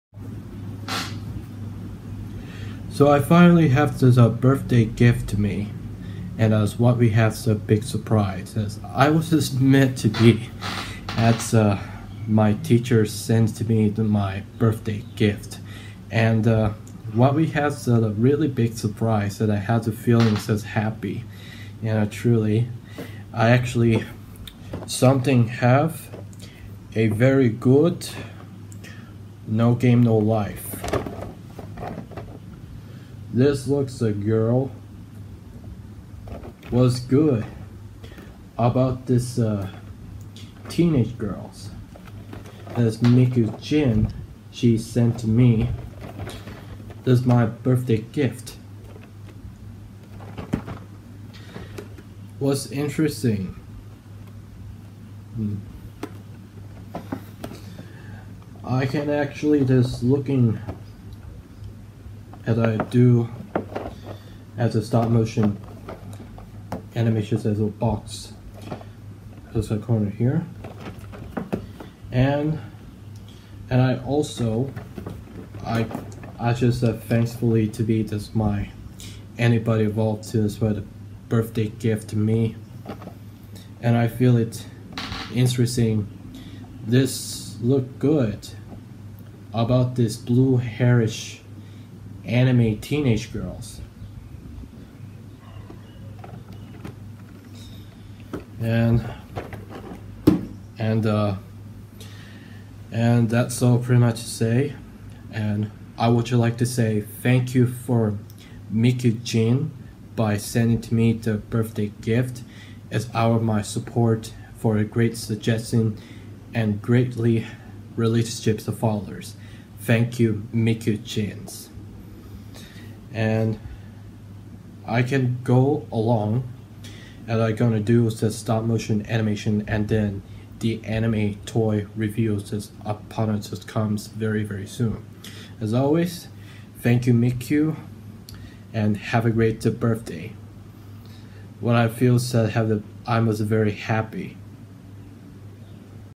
To Miku-Chan Thanks To Wished My Birthday Gift
Just about this time I was turned young adult are 20 year old was very good about birthday gift like Anime Toys.
I’ll do get making Stop Motion Animation And Anime Toy Reviews